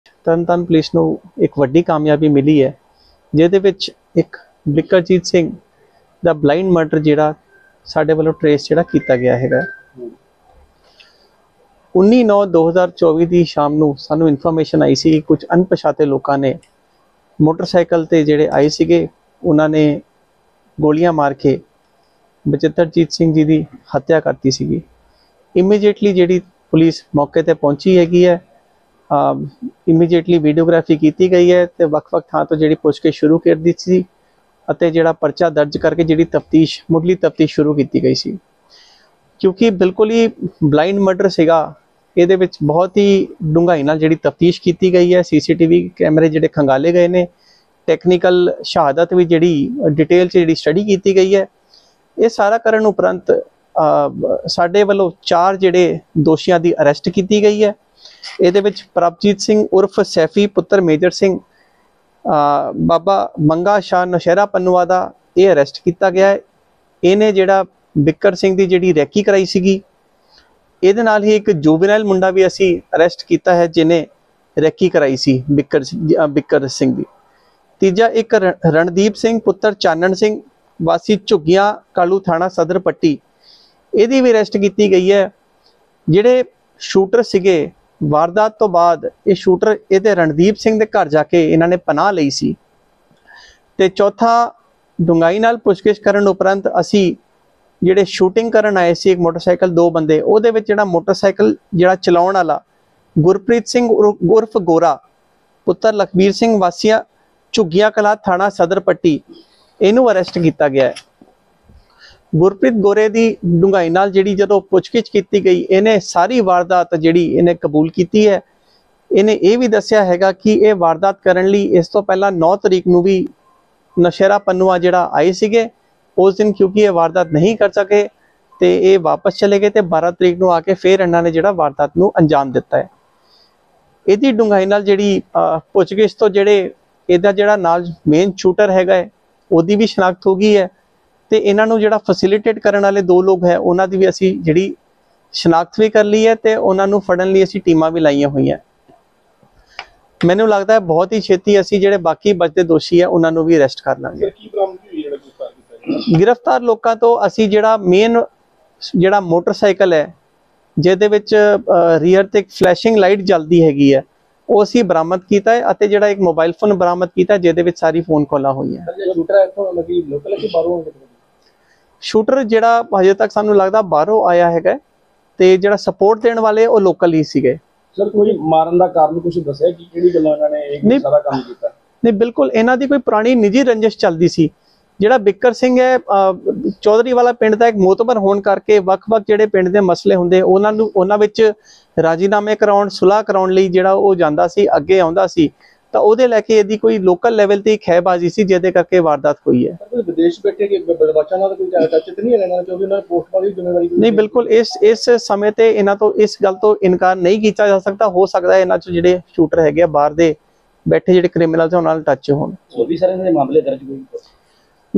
AAP ਵਰਕਰ ਦੇ ਕਤਲ ਮਾਮਲੇ 'ਚ 4 ਮੁਲਜ਼ਮ ਗ੍ਰਿਫ਼ਤਾਰ, ਗੁੱਝੇ ਭੇਤ ਆਏ ਸਾਹਮਣੇ | Patti News |Panchayat Elections
AAP ਵਰਕਰ ਦੇ ਕਤਲ ਮਾਮਲੇ 'ਚ ਪੁਲਿਸ ਨੇ 4 ਮੁਲਜ਼ਮ ਕੀਤੇ ਗ੍ਰਿਫ਼ਤਾਰ। ਮੁੱਖ ਸ਼ੂਟਰ ਹਾਲੇ ਵੀ ਗ੍ਰਿਫ਼ਤ ਤੋਂ ਬਾਹਰ, ਵਿਦੇਸ਼ ਬੈਠੇ ਗੈਂਗਸਟਰ ਨੇ ਲਈ ਜ਼ਿੰਮੇਵਾਰੀ।